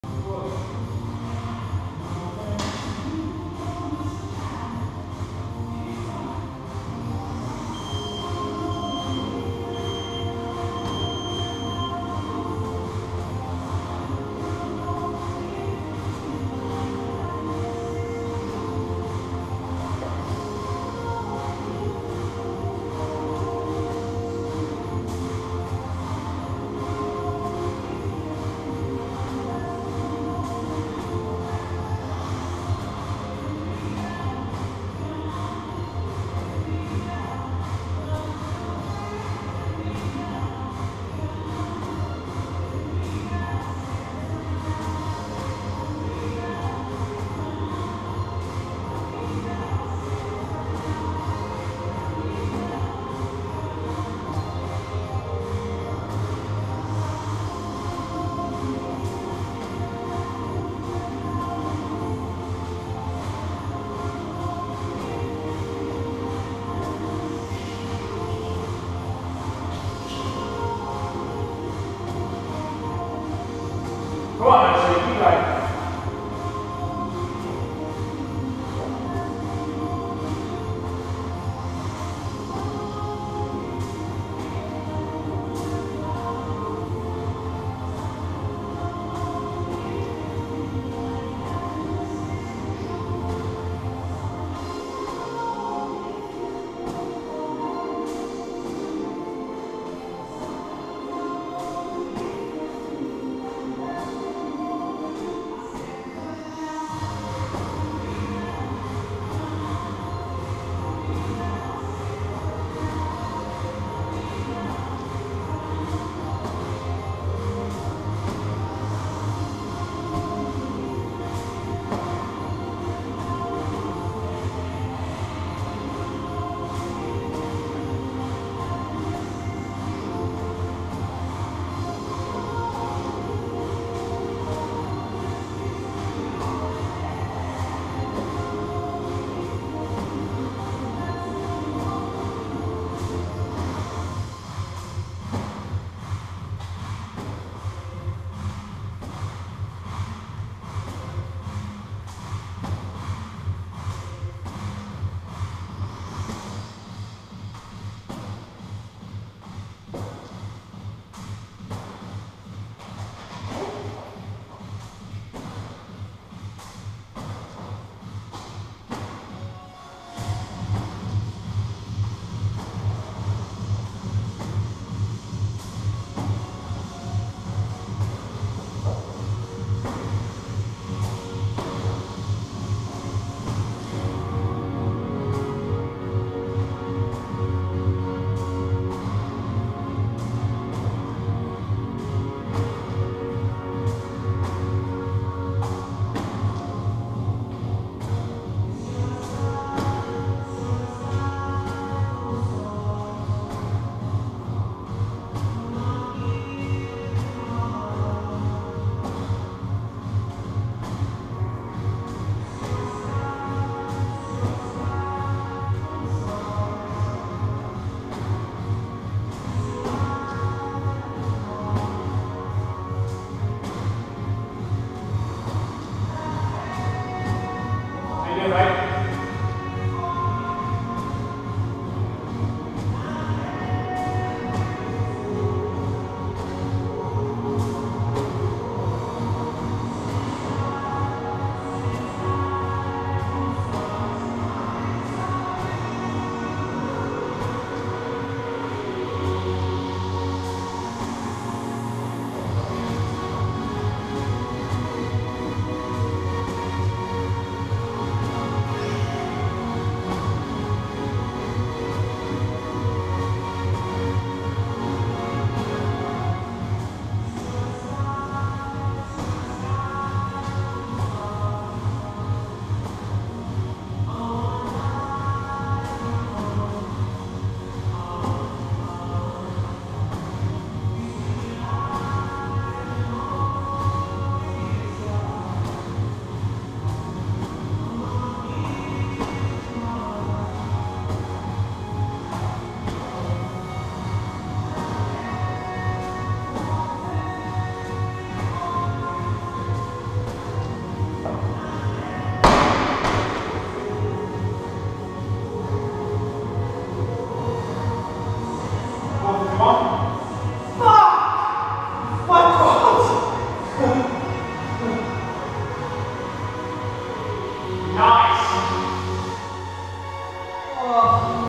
Ollie Campbell doing Karen UNBROKEN in 4.29 Priority 6 Home of Crossfit Oxford Originals
OC doing Karen- 150 wall balls unbroken in 4.29... actually he does 163 unbroken as he wanted to make sure he hit the 150, clock says 4.29 at 150 reps. is it a world record?